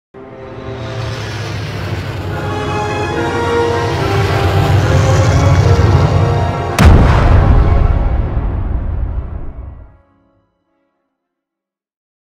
Star Wars CGI | Tantive IV
Our old CGI render test for star wars short film.